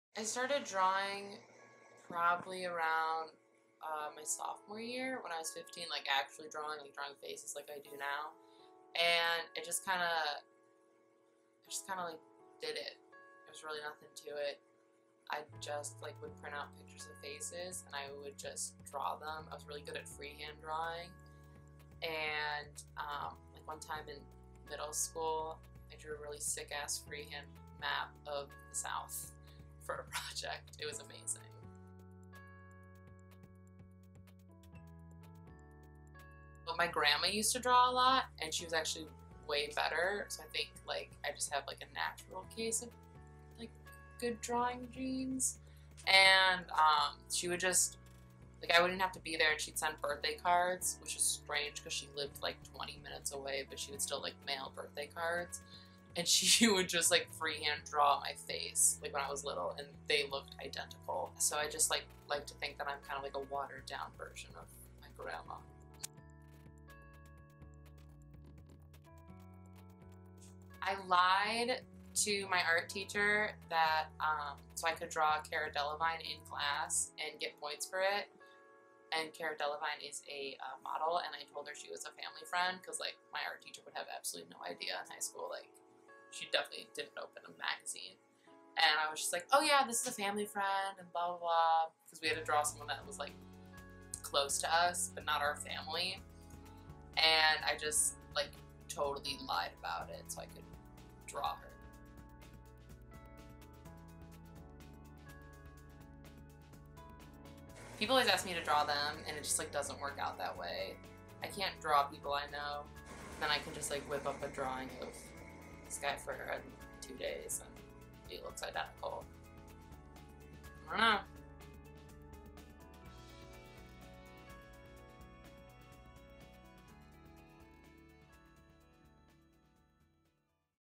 Sarah Weingarten Discusses Visual Sketching
Ohio University Freshman Sarah Weingarten talks about her hidden talent, sketching faces from pictures. Shot and edited by Marlena Scott.